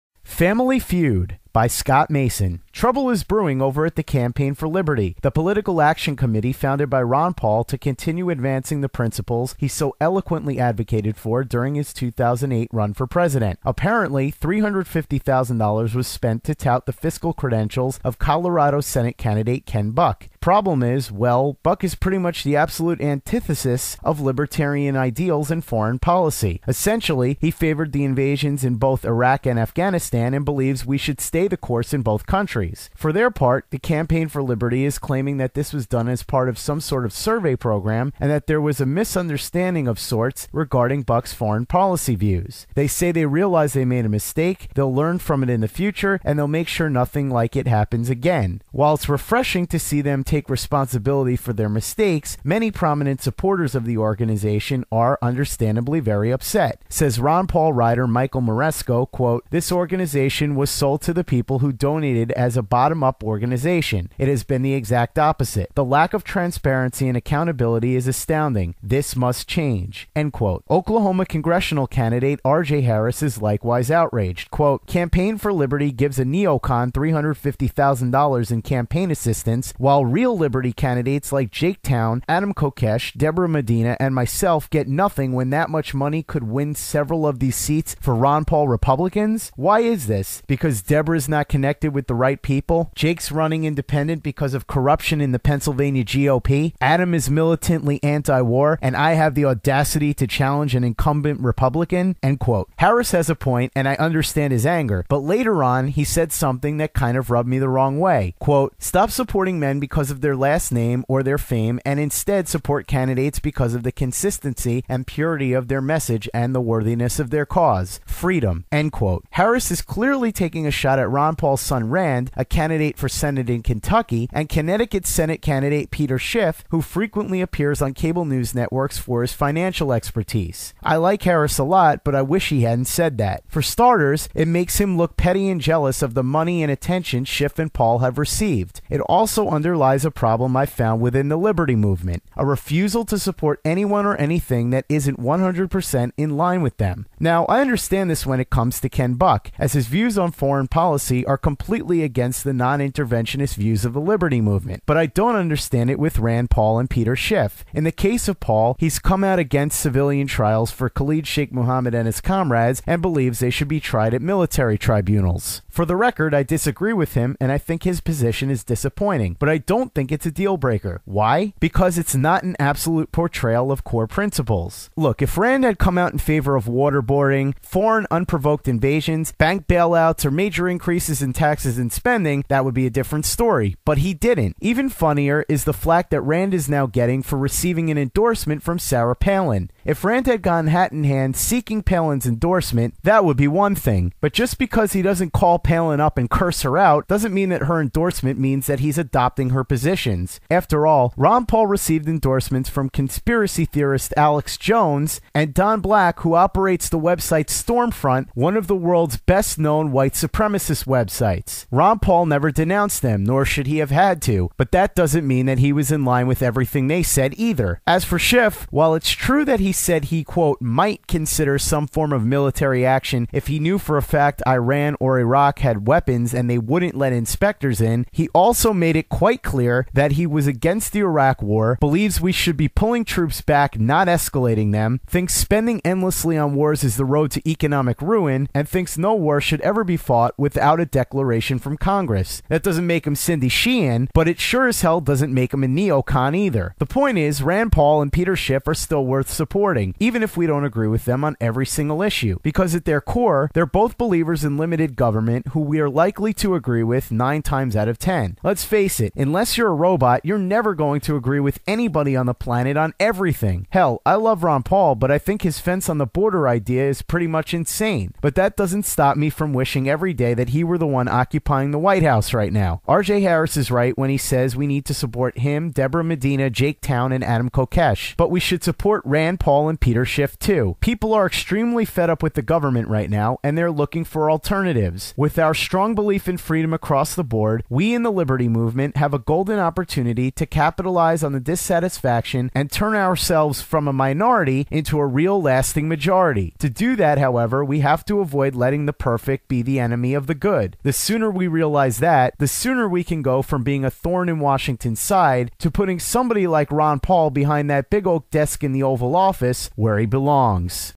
Campaign For Liberty's Family Feud
Scott Mason's audio blog for 2/2/2010 looks at the infighting at The Campaign for Liberty and why the Liberty movement needs to stop taking such a hard line stance on every issue to advance their cause and end the "Family Feud."